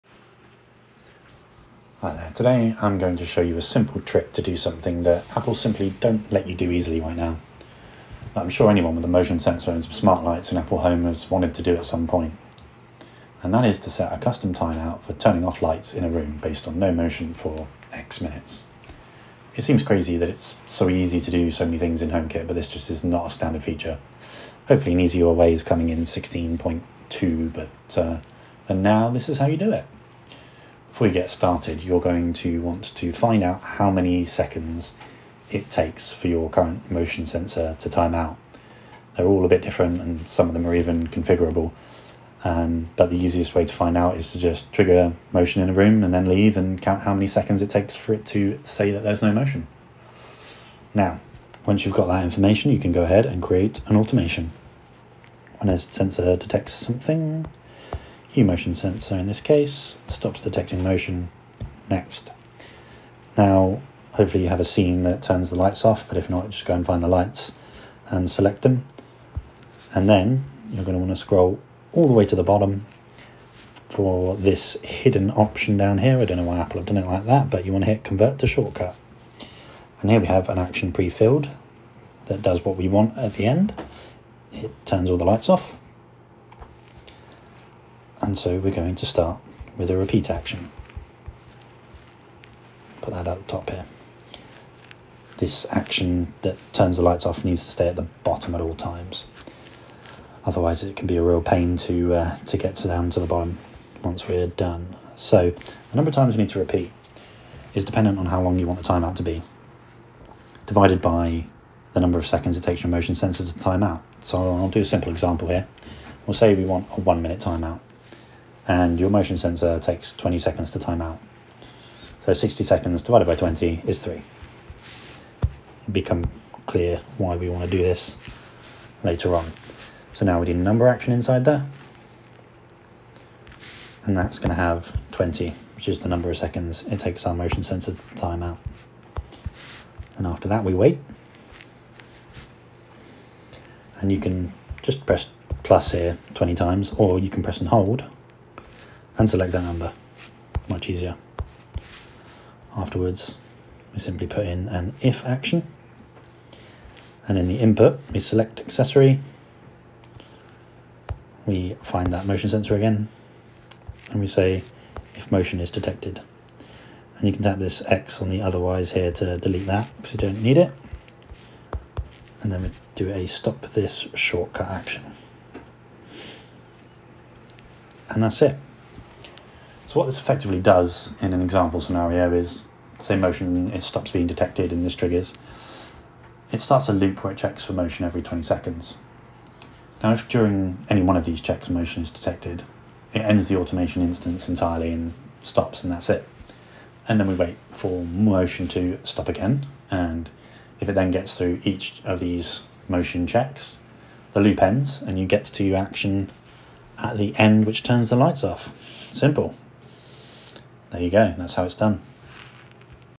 The way to add custom motion timeout for HomeKit lights Apple doesn't tell you about
Apple doesn't officially give you any way to turn off lights after a specified duration of motion inactivity in HomeKit, but there is a way!